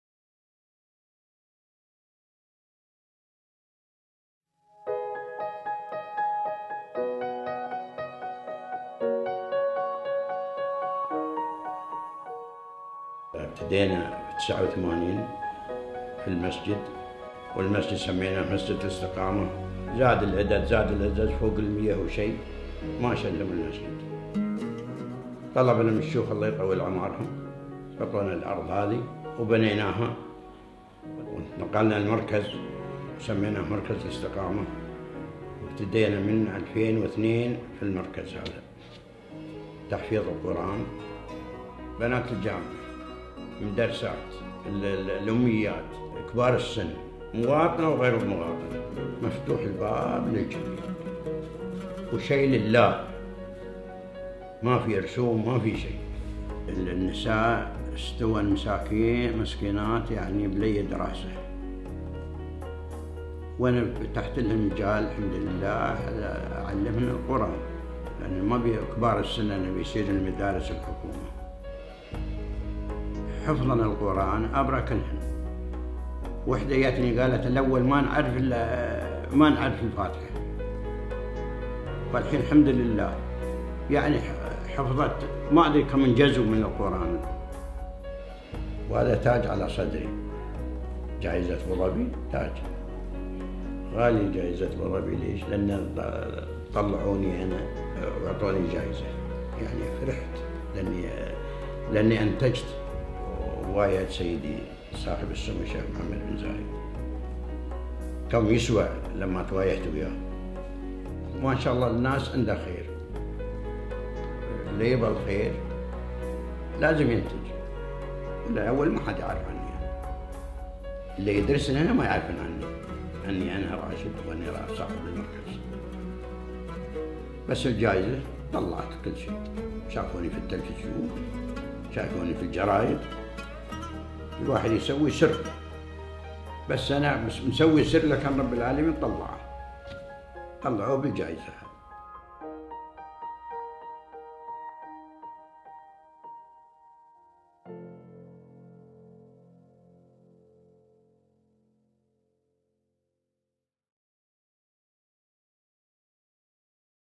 Rashid Mohammed Al Dhaheri
The story of Abu Dhabi Awards recipient Rashid Mohammed Al Dhaheri